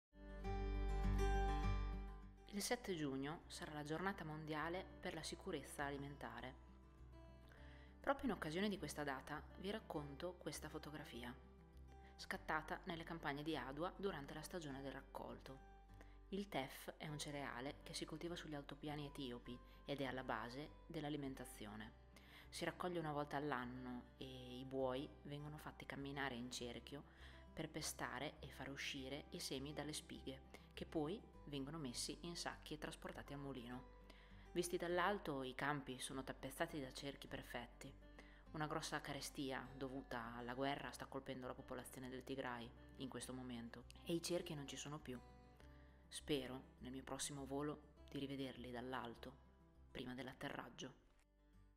Il raccolto
Sedicesima puntata della rubrica "Cartoline da Adwa".
La "cartolina" di oggi è dedicata alla Giornata mondiale per la sicurezza alimentare. Carolina ci accompagna nelle campagne di Adwa durante la stagione del raccolto.